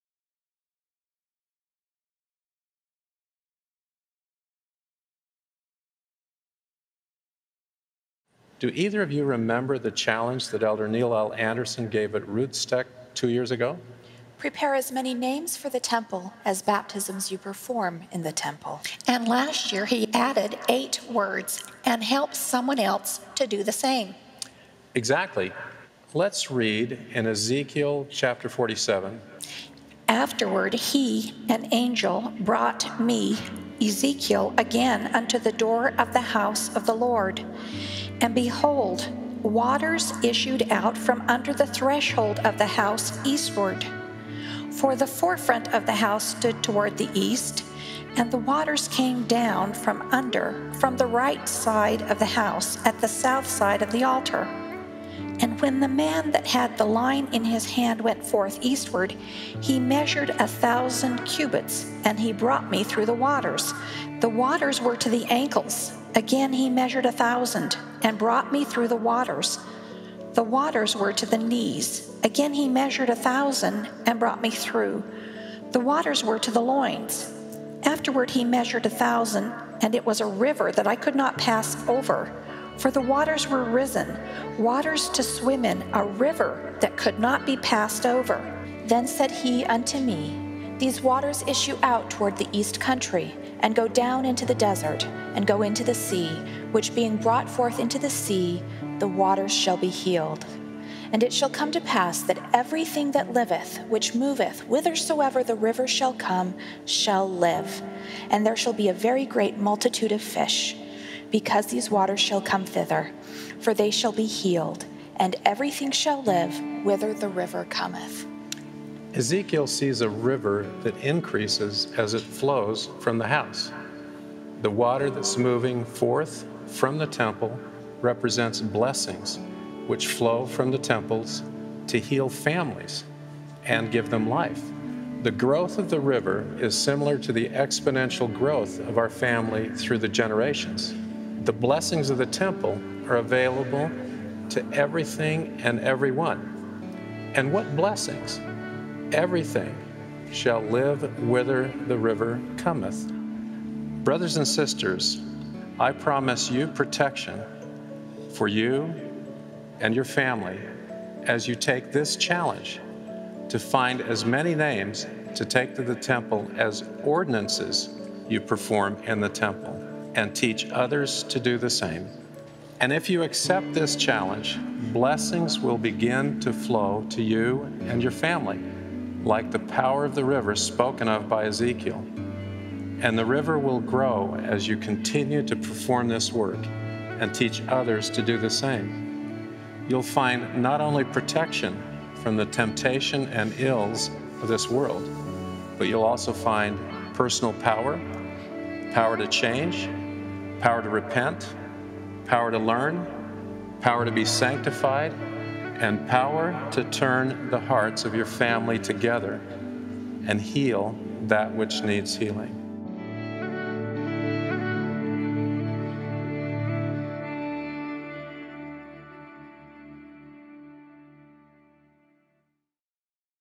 OT 44.2 And the River Will Grow
In this video Elder Renlund and his family explain the significance and meaning behind the temple river vision in Ezekiel and how we can become part of the river in our own temple worship and family history work.